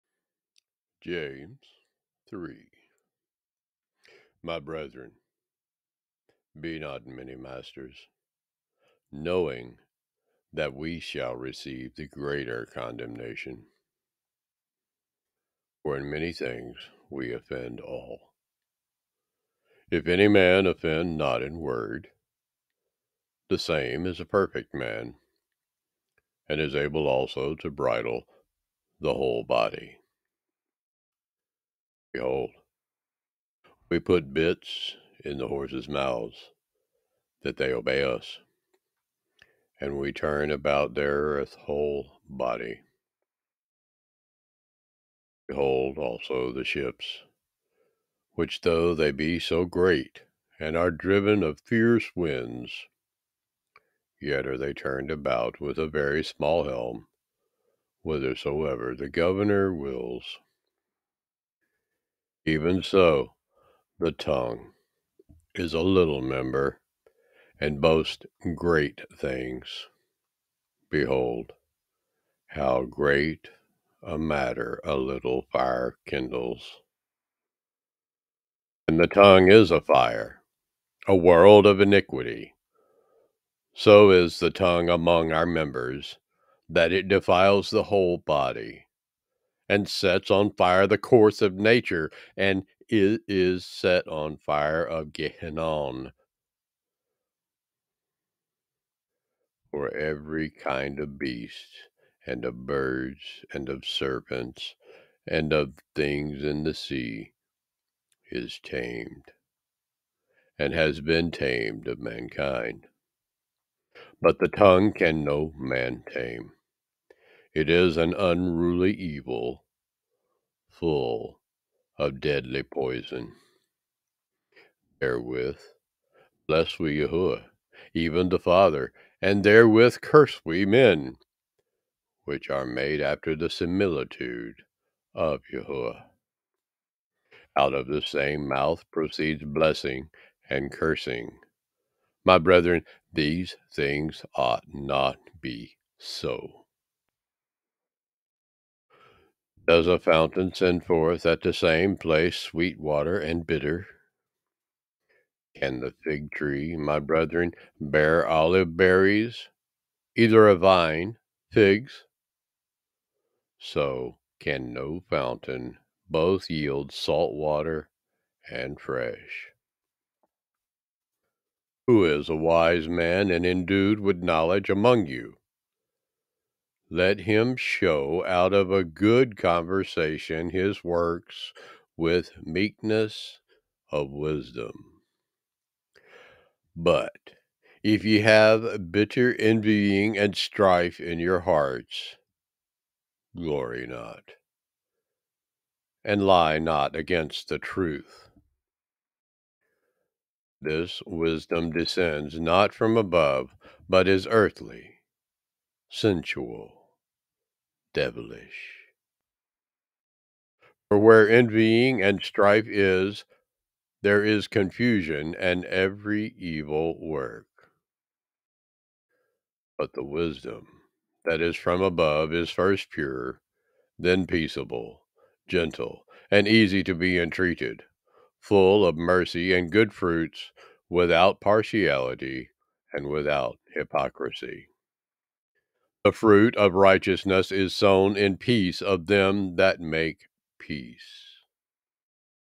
James - Ya'aqov 3 et Cepher
James - Ya'aqov 3 Read by Richard Lynch from the et Cepher Bible Version
All Glory to YAHUAH throught His Son YAHUSHA